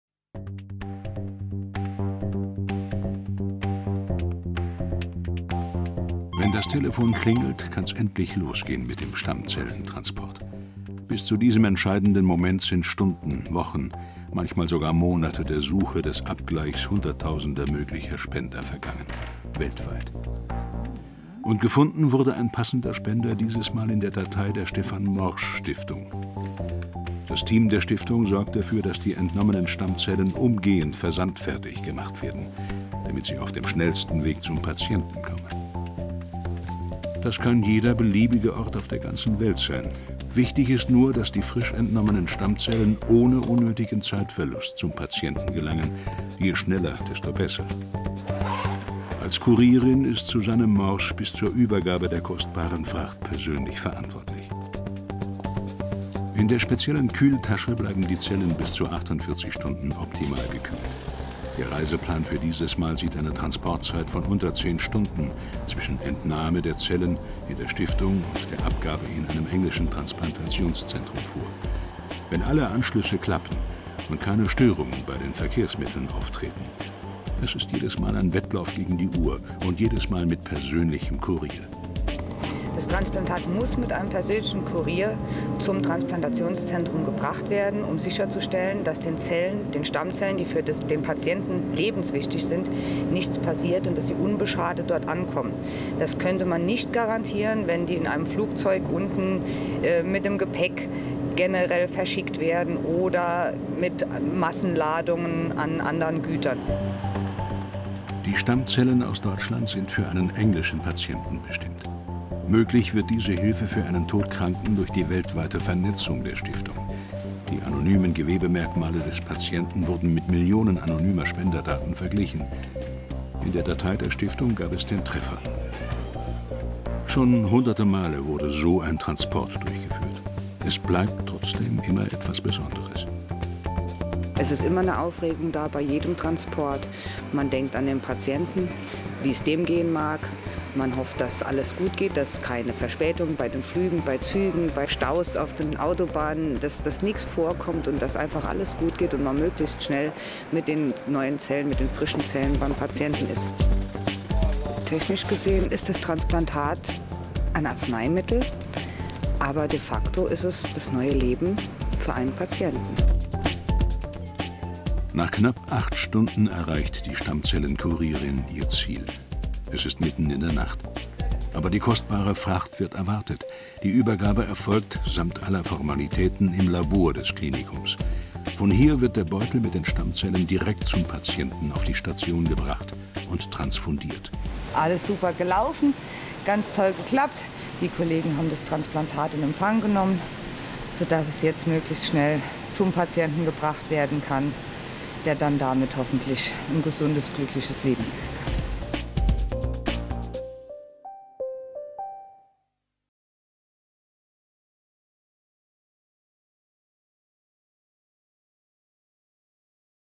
Auftrag des Lebens: Unterwegs mit dem Stammzellenkurier der Stefan-Morsch-Stiftung
Susanne Morsch leitet normalerweise die Spenderdatei der Stefan-Morsch-Stiftung. Doch fünf bis sechs Mal im Jahr tauscht die Sozialfachwirtin ihren Computer gegen eine Kühlbox, die sie hütet wie ihren Augapfel. Denn dann ist sie Kurierin - Stammzellkurierin. Und das ist etwas ganz Besonders. Denn dann überbringt sie so schnell als möglich - innerhalb von 72 Stunden - das Transplantat zu einem Leukämie- oder Tumorpatienten, der ohne diese blutbildenden Stammzellen keine Chance auf Überleben hat. Und Susanne Morsch reist dafür rund um den Globus. Bei einer dieser Touren hat sie ein Filmteam begleitet: Ihre Reise geht nach Großbritannien - und schreibt ein neues Kapitel in der Geschichte eines englischen Patienten.

Die Stefan-Morsch-Stiftung betreibt nicht nur Deutschlands älteste Stammzellspenderdatei. Die der hauseigenen Entnahmestation werden Stammzellen entnommen, die dann auf dem schnellsten Weg zu den Transplantationskliniken gebracht werden müssen, wo der Leukämiepatient auf das lebensrettende Transplantat wartet.

Copyright by Diogenes Filmproduktion